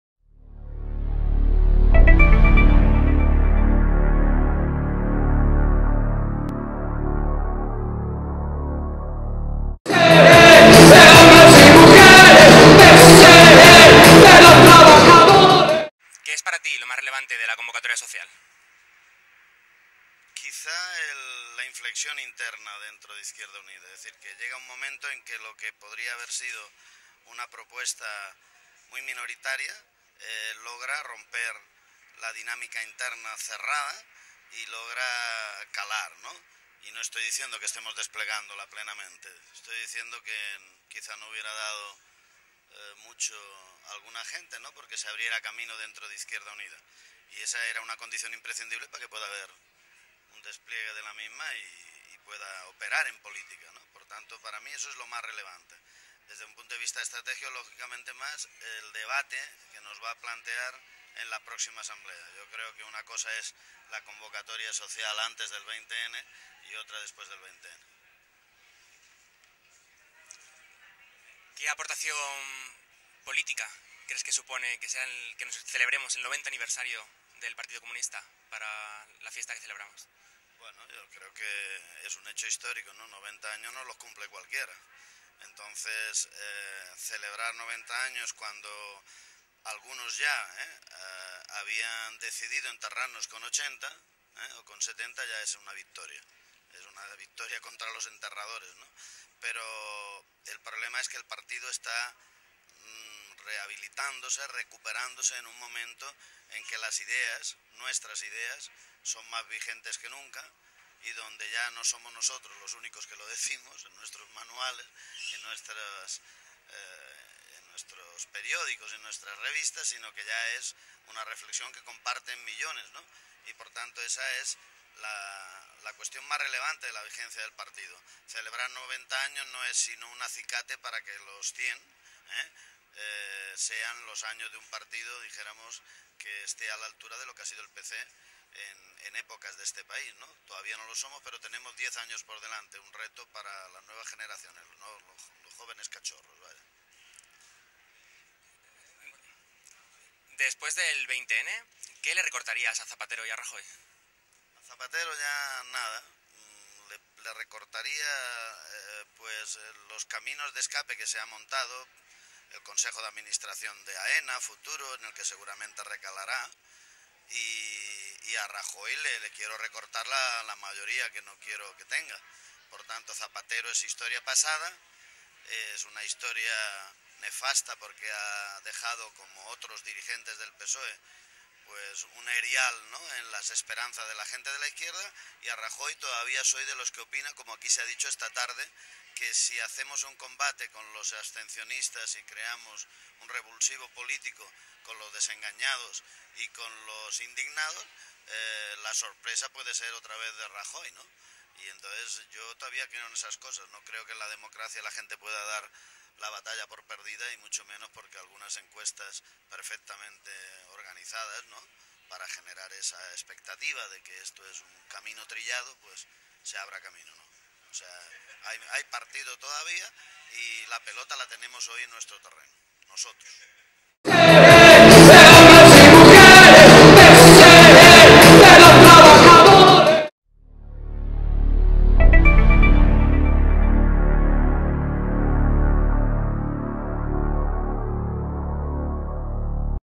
Entrevista a Ramón Luque. Fiesta PCE 2011
Entrevista a Ramón Luque, coordinador de campaña electoral de IU, en la fiesta del PCE.